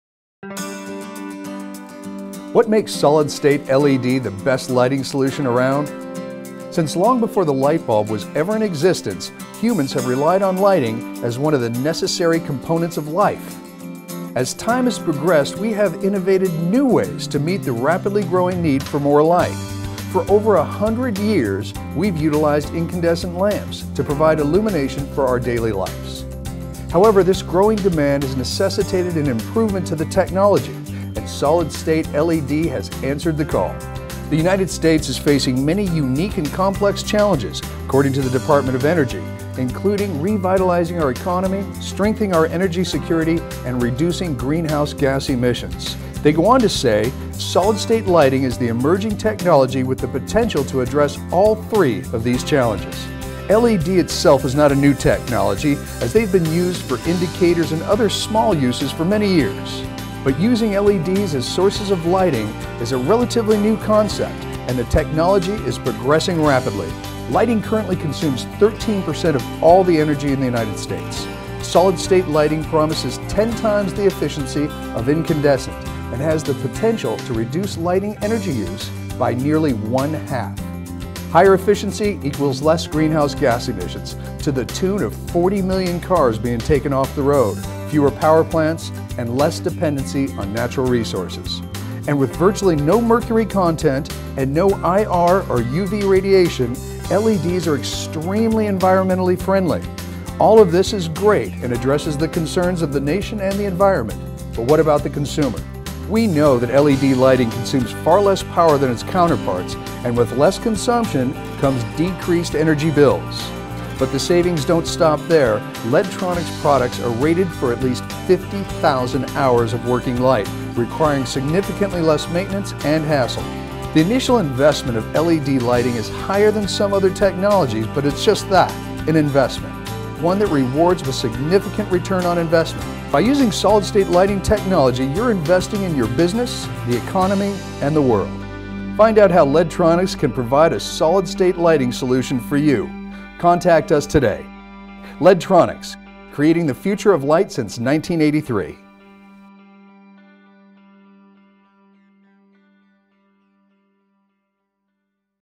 Light Viewing Episode 1: What Makes Solid State LED the Best Lighting Solution Around?
Since long before the light bulb was ever in existence humans have relied on lighting as one of the necessary components of life. As time has progressed we have innovated new ways to meet the rapidly growing need for more light. For over 100 years we have utilized incandescent lamps to provide illumination for our daily lives. However this growing demand has necessitated an improvement to the technology and solid state LED has answered the call.
 The United States is facing many unique and complex challenges, according to the Department of Energy, including "revitalizing our economy, strengthening our energy security, and reducing greenhouse gas emissions." They go on to say that "solid state lighting is an emerging technology with the potential to address all three of these challenges.
 LED itself is not a new technology, as they have been used for indicators and other small uses for many years. But using LEDs as sources of lighting is a relatively new concept, and the technology is progressing rapidly. Lighting currently consumes 13% of all energy in the United States. Solid state lighting promises ten times the efficiency of incandescent, and has the potential to reduce lighting energy use by nearly one half.
Higher efficiency equals less greenhouse gas emissions to the tune of 40 million cars being taken off the road, fewer power plants, and less dependency on natural resources. And with virtually no mercury content and no IR or UV radiation, LEDs are extremely environmentally friendly. All of this is great, and addresses the concerns of the nation and the environment...but what about the consumer?
We know that LED lighting consumes far less power than its counterparts, and with less consumption comes decreased energy bills. But the savings don't stop there. LEDtronics products are rated for at least 50,000 hours of working light, requiring significantly less maintenance and hassle.
The initial investment of LED lighting is higher than some other technologies, but it is just that: an investment, but one that rewards with a significant ROI. By using solid state lighting technology you are investing in your business, the economy, and the world.